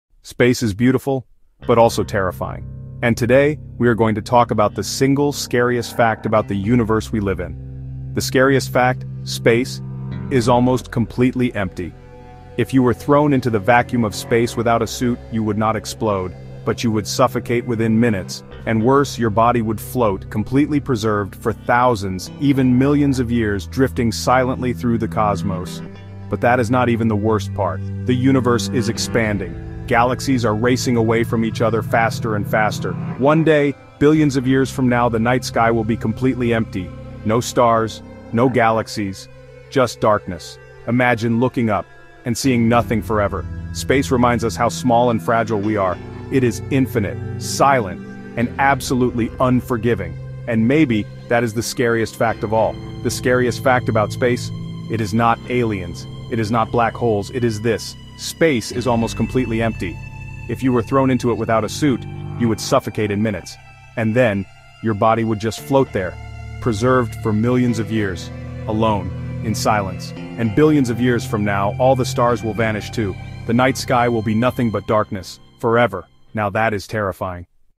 🌌 The Scariest Fact About Space | Why the Universe Is Terrifying
Space is beautiful… but also terrifying. The scariest fact? Space is almost completely empty. If you were thrown into the vacuum of space without a suit, you wouldn’t explode—but you’d suffocate within minutes. Your body would then drift silently through the cosmos, preserved for thousands—or even millions—of years.

And that’s not all. The universe is expanding, with galaxies moving away from each other faster and faster. Billions of years from now, the night sky may be completely empty—no stars, no galaxies, just eternal darkness.

📌 In this video, you’ll discover:

Why space is almost entirely empty

What would happen if a human were exposed to a vacuum

The terrifying future of the expanding universe

Why the scariest fact about space isn’t aliens or black holes—it’s the emptiness and silence